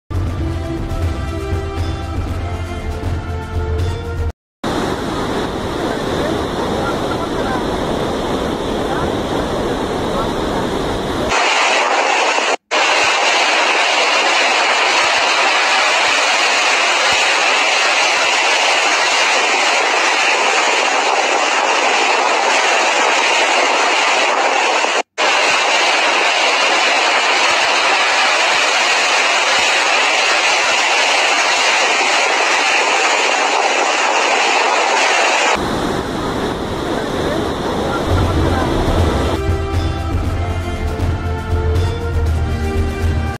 Mosa nadi hai allowed report channel 9 news
Hyderabad nadi Musa nadi hi allowed water hai report channel 9 news Hyderabad Telangana India continue report